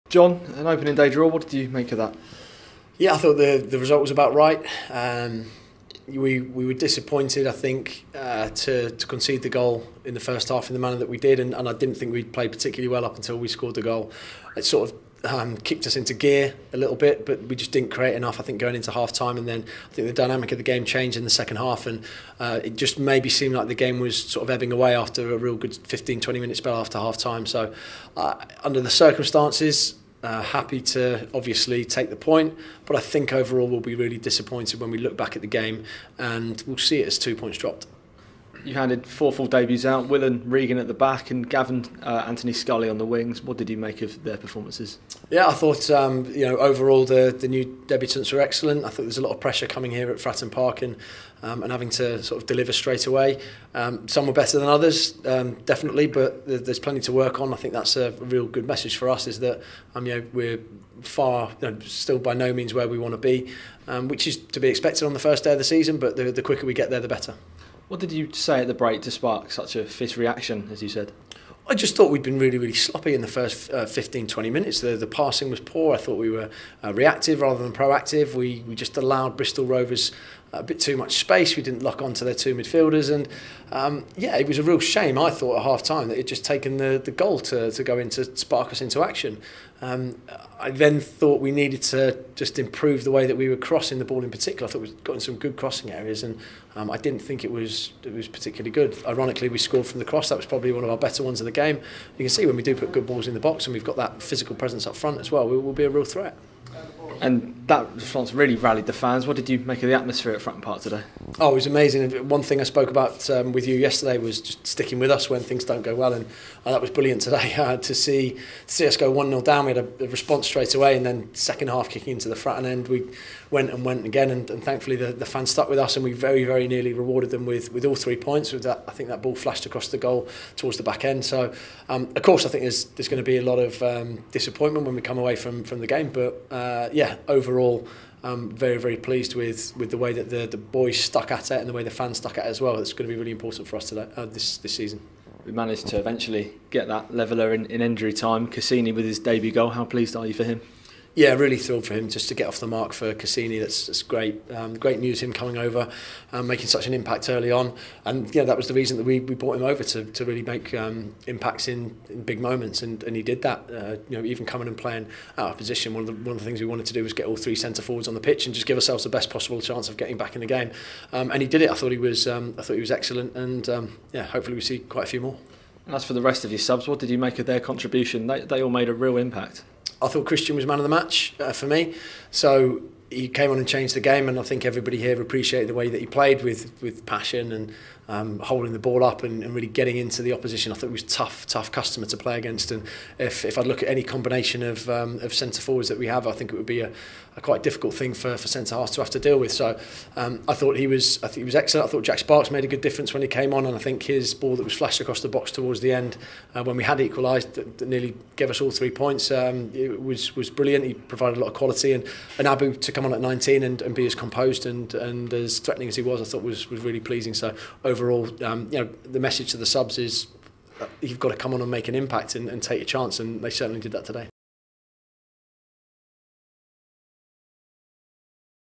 John Mousinho post-match | Pompey 1-1 Bristol Rovers
Head coach John Mousinho talks after Pompey's opening day draw with Bristol Rovers.